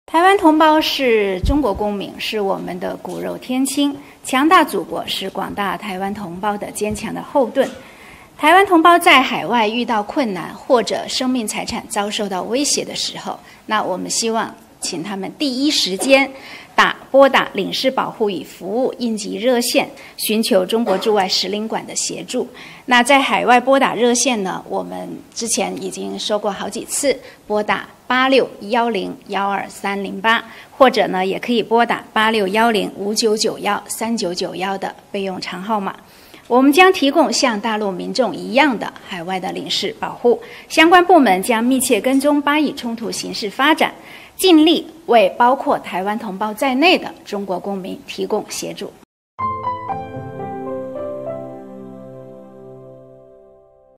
大陆会对巴以当地台胞提供哪些协助？国台办回应
国台办发言人朱凤莲10月25日表示，台湾同胞在海外遇到困难，或受到生命财产威胁时，请第一时间拨打领事保护与服务应急热线寻求中国驻外使领馆协助。　　25日，国台办举行例行新闻发布会。有记者提问：据台媒报道，一名滞留以色列的台湾民众在社交媒体发文披露，原想寻求民进党当局协助却遭冷漠答复“请上网订机票”。有台湾网民表示，应求助中国大陆使馆。对此有何评论？请问大陆对巴以当地台胞或有意撤离的台胞会提供哪些协助？　　对此，朱凤莲表示，台湾同胞是中国公民，是我们的骨肉天亲，强大祖国是广大台湾同胞的坚强后盾。台湾同胞在海外遇到困难，或受到生命财产威胁时，请第一时间拨打领事保护与服务应急热线寻求中国驻外使领馆协助(在海外拨打热线的方式是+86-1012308，也可拨打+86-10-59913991的备用长号码)。我们将提供像大陆民众一样的海外领事保护。相关部门将密切跟踪巴以冲突形势发展，尽力为包括台湾同胞在内的中国公民提供协助。